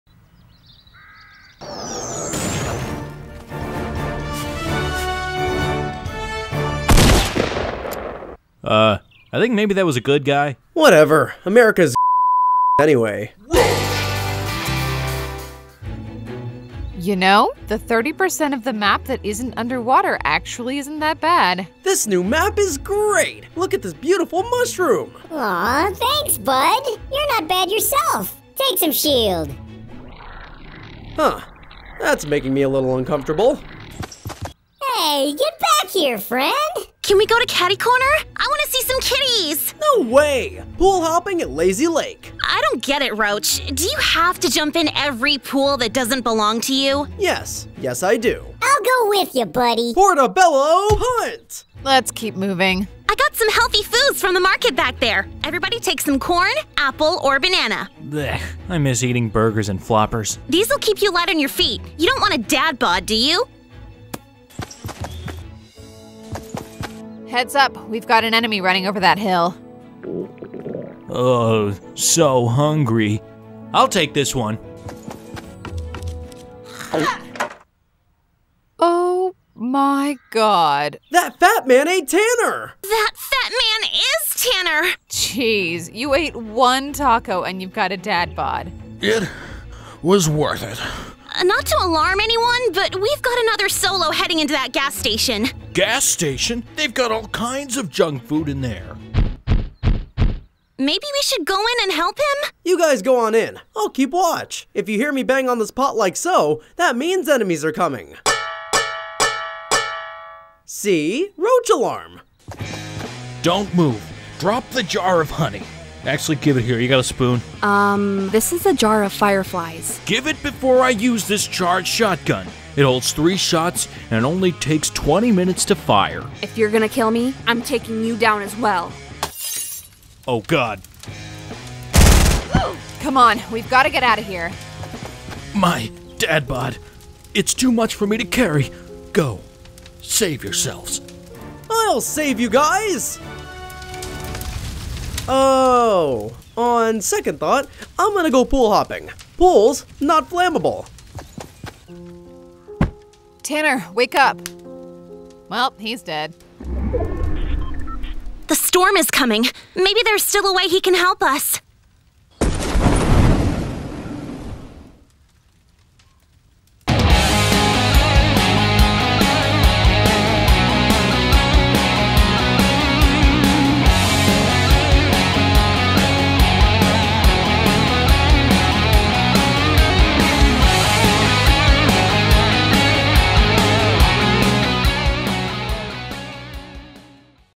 Jules & The Gas Station | The Squad (Fortnite Animation)
Fortnite animation with Jules and Dad Bod Jonesy at the Gas N Grub flammable gas station! Watch out for Firefly Jar, Lada Taco Guaco Skin. Captain America makes an appearance too!

Tanner - Jonah Rashid
Roach - Austin Cook
Anya - Marianne Bray
Caroline - Elsie Lovelock
Jules - Allison Seils
Animated by Studio 84